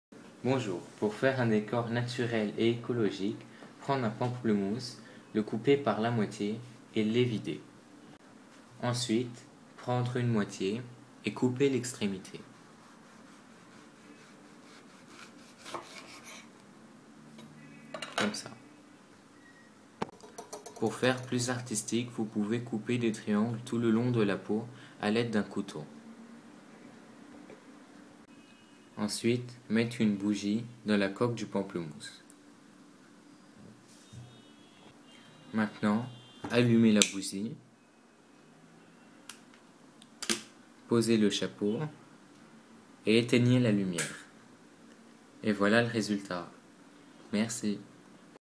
Avoir idée décoration maison - Faire des photophores maison - Bougeoirs fruits
Avoir idée décoration maison est souvent très simple. Par exemple, vous pouvez faire des bougeoirs avec des pamplemousse pour éclairer la maison. Idéal pour amuser les enfants.